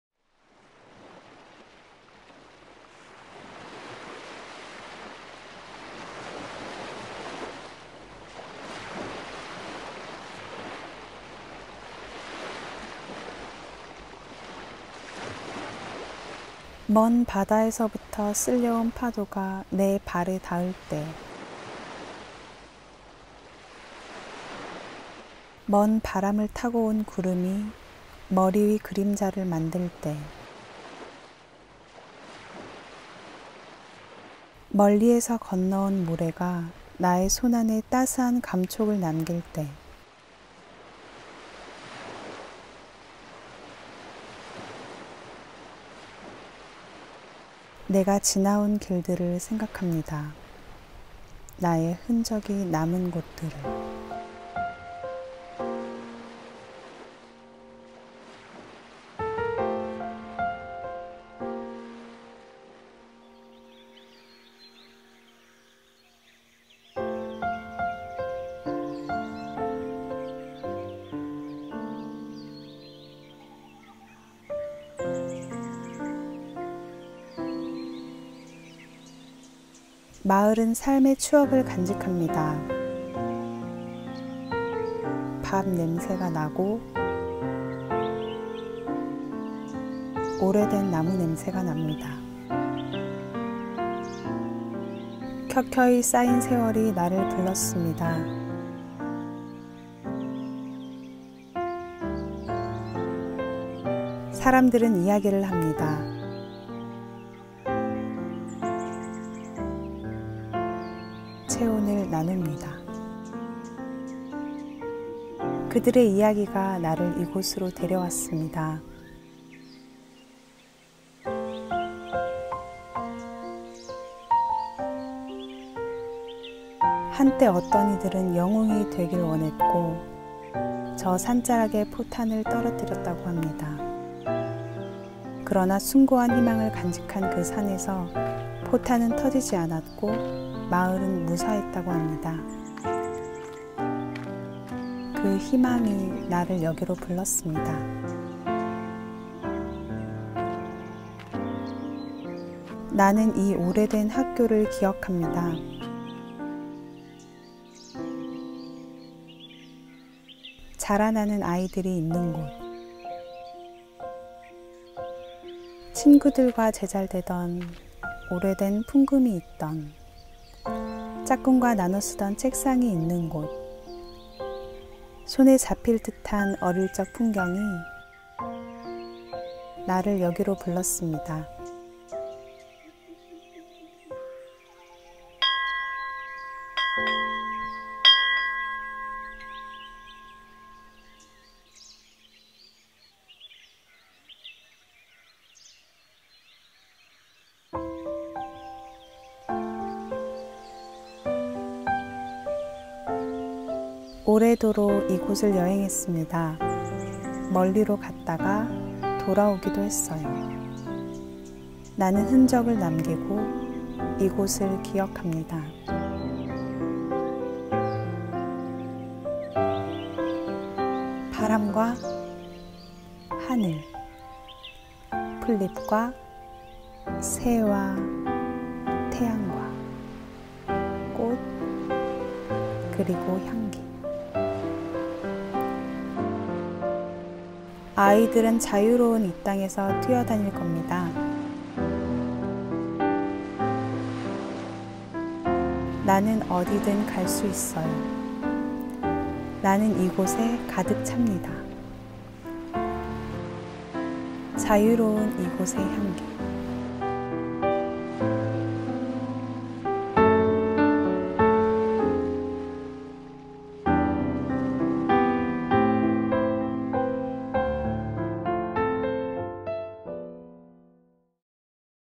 [Plug in DMZ] 향기
6·25전쟁이 비껴간 동해안 최북단 마을
왕곡마을.
마을은 삶의 추억을 간직합니다.